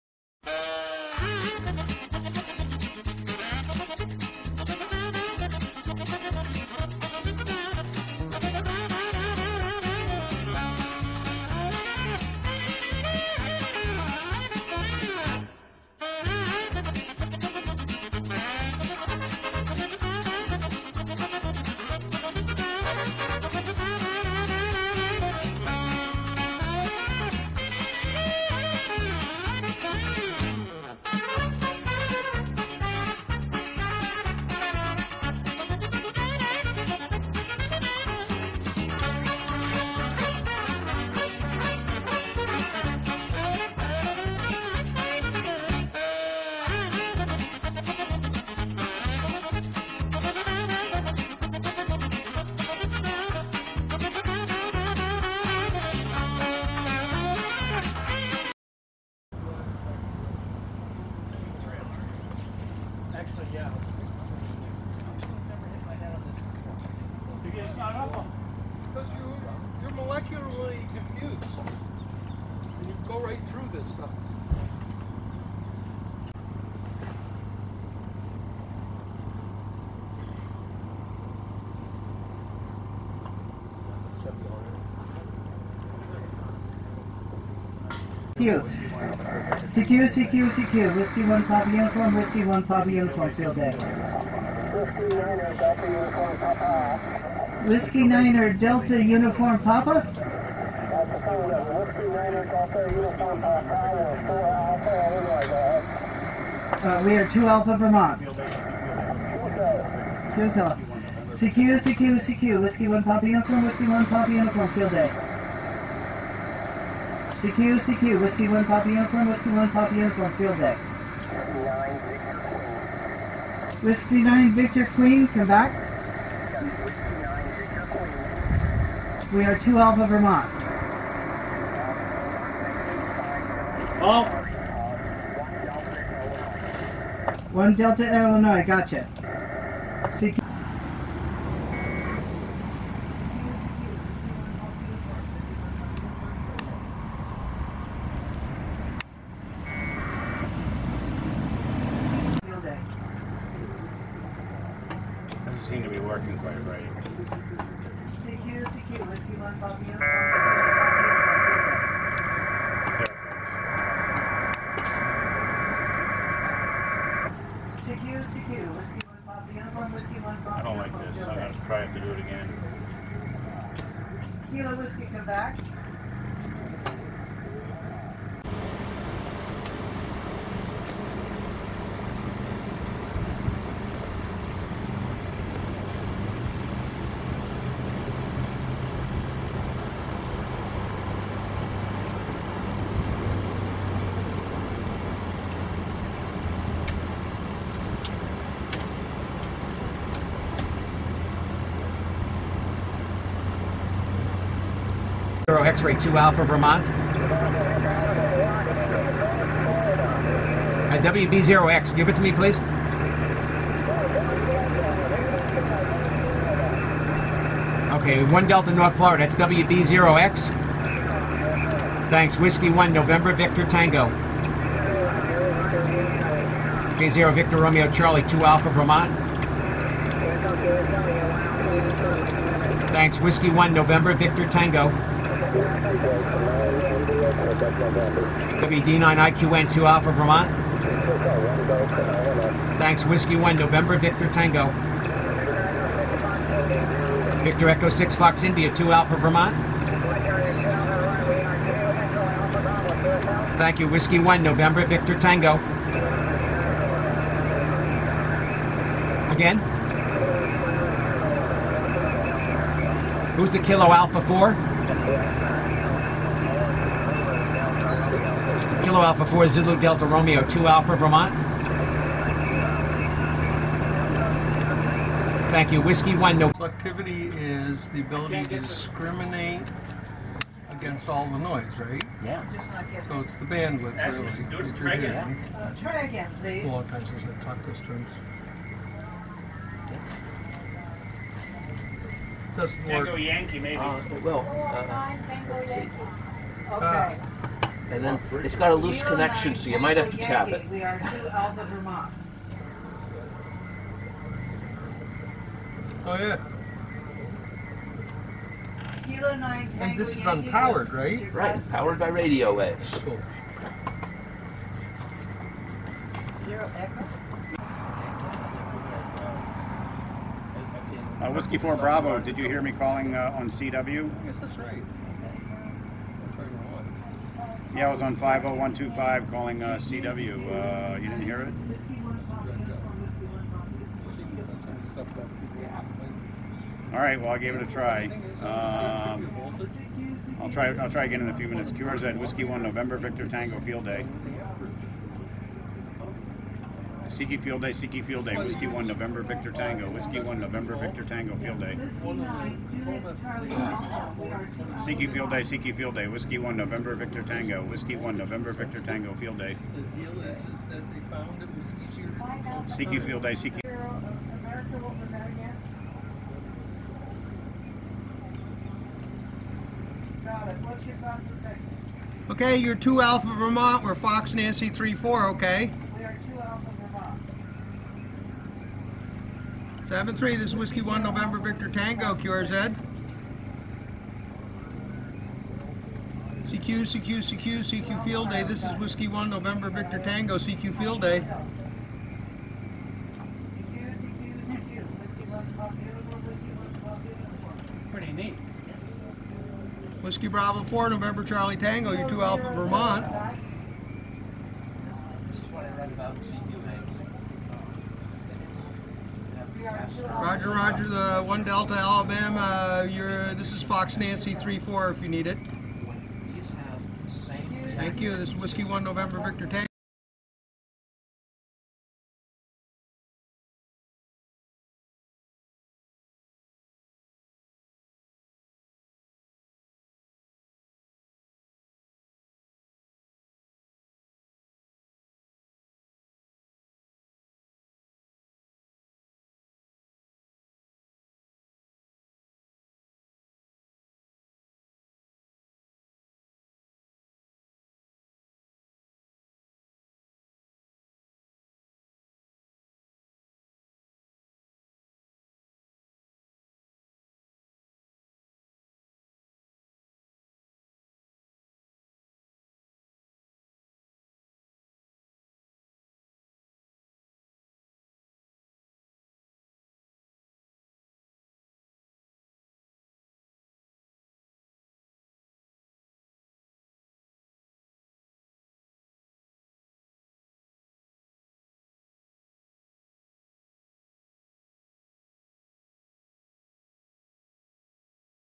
Field Day in Vermont
Radio Amateurs of Northern Vermont (RANV) on ARRL field day, 2017.
First is the tower/yagi setup for the GOTA/VHF, UHF stations.  Then operations from GOTA, CW, Phone and VHF, as well as a demonstration of a crystal radio.  Finally, the tear down of the large tent.